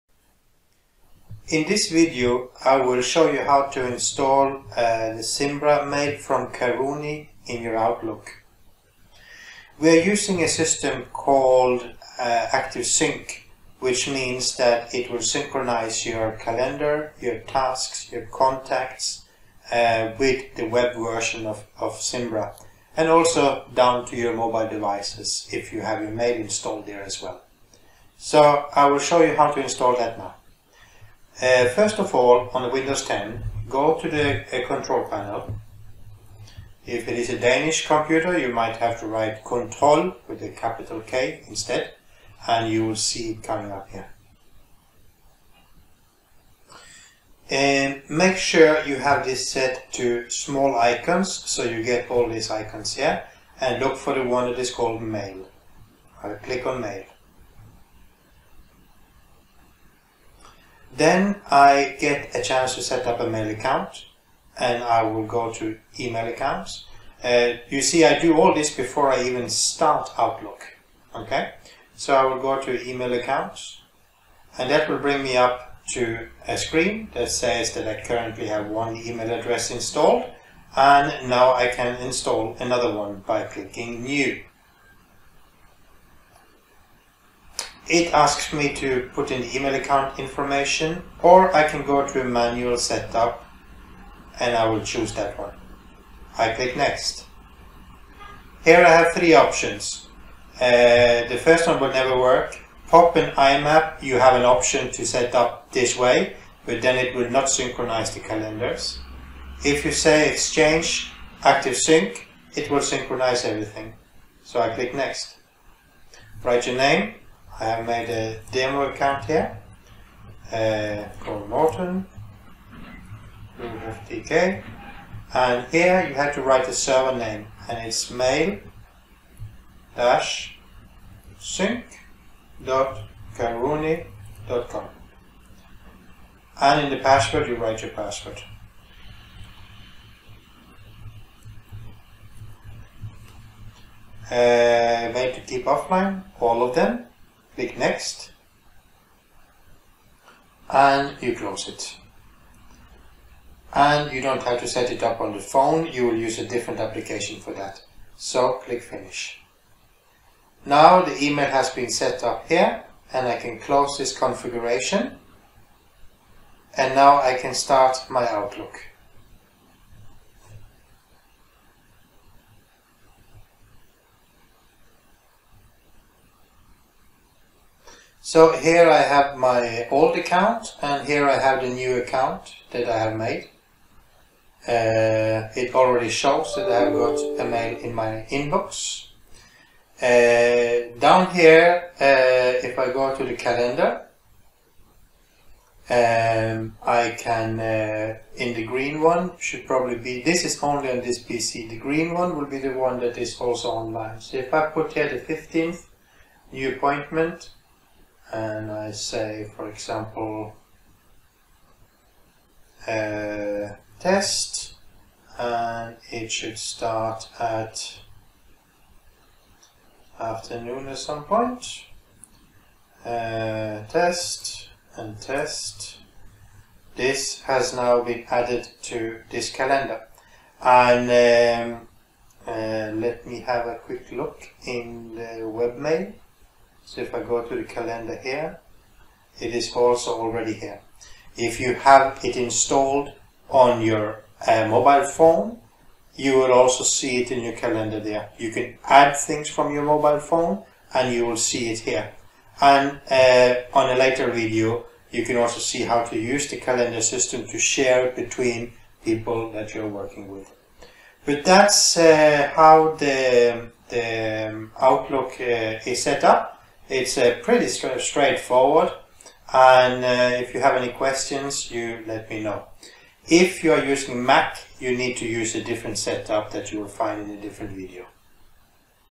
Zimbra Mail Setup in Outlook | Active Sync
How to setup your your mail from Karuny into your outlook in a way that it will synchronize mails, contacts and calendars